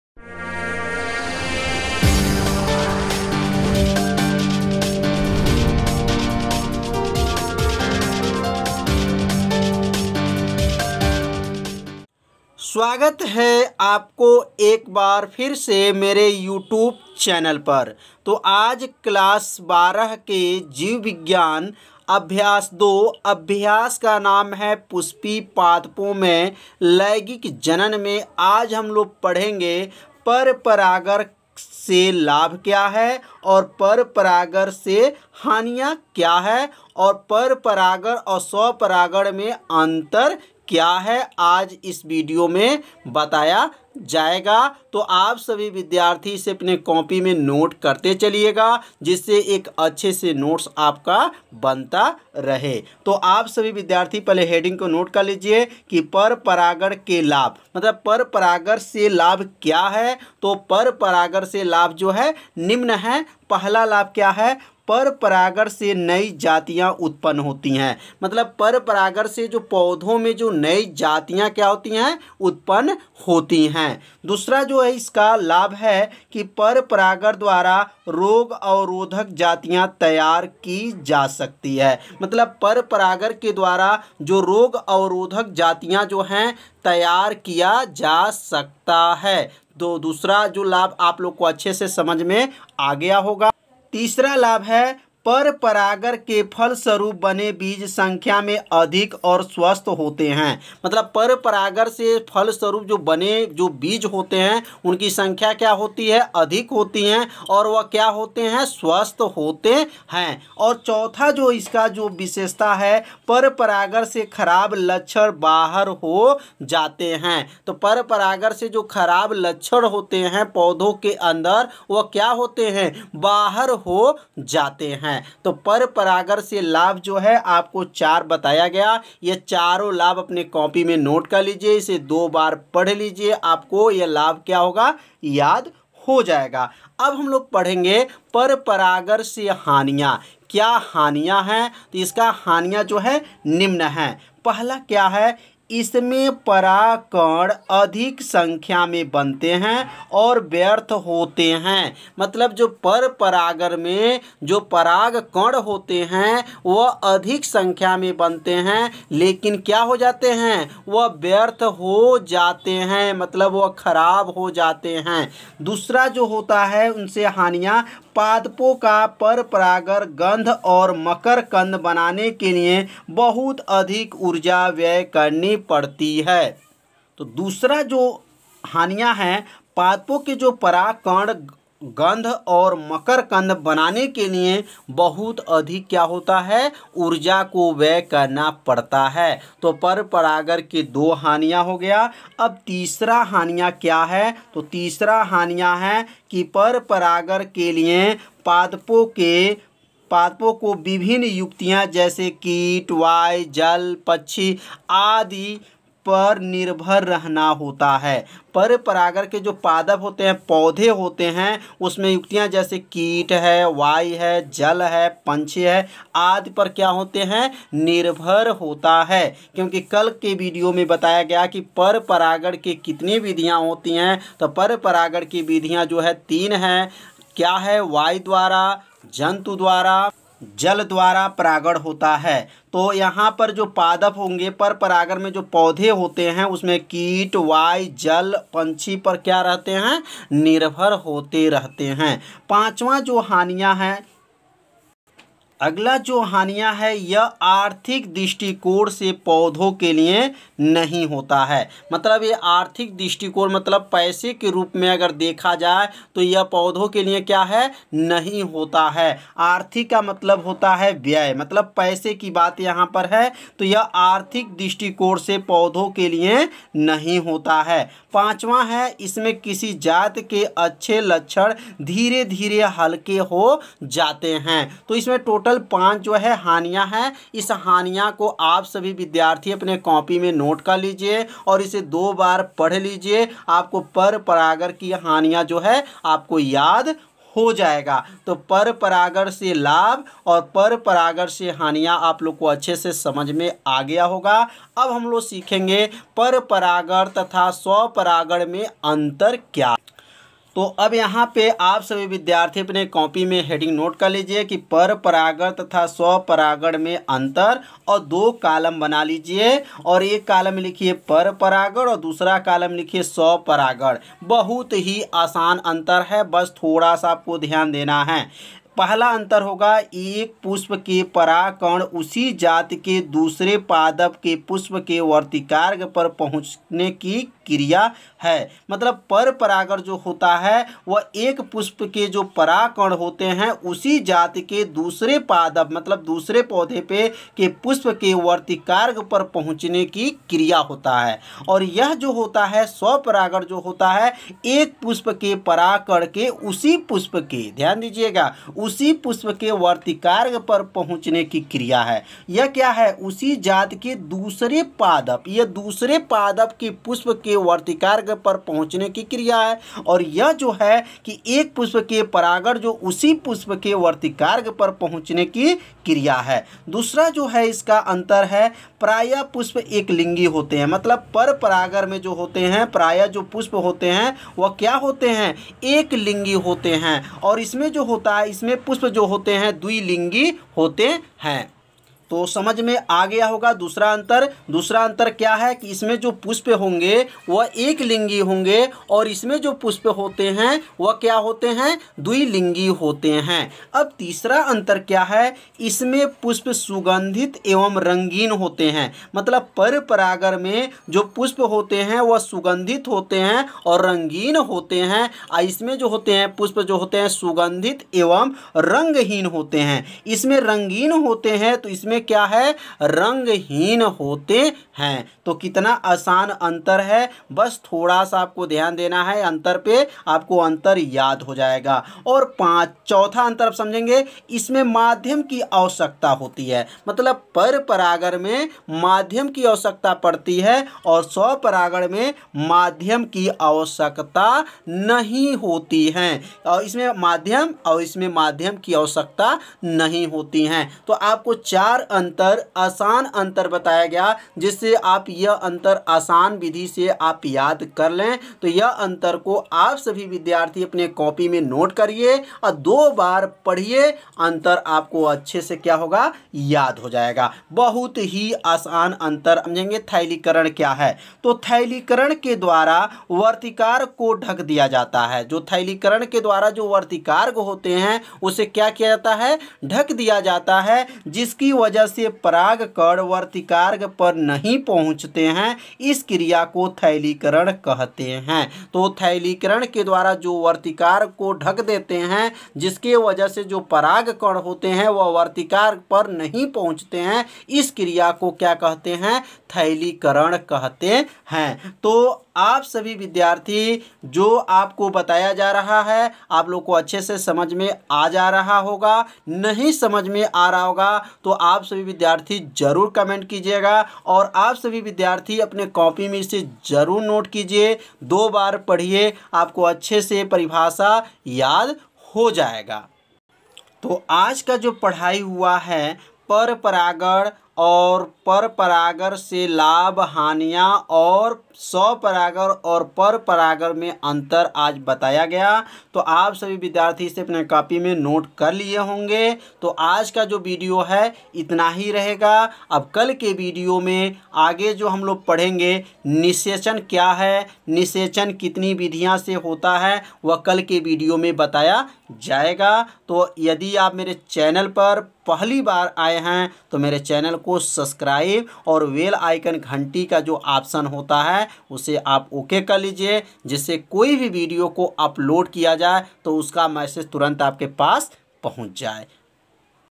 Class 12 biology chapter-2 परपरागण से लाभ तथा हानियां परपरागण तथा स्वपरागण में अन्तर part-7 rastogi
Class 12 biology chapter-2 पुष्पी पादपों में लैंगिक स्वपरागण से लाभ तथा हानियां पर परागण वायु part 6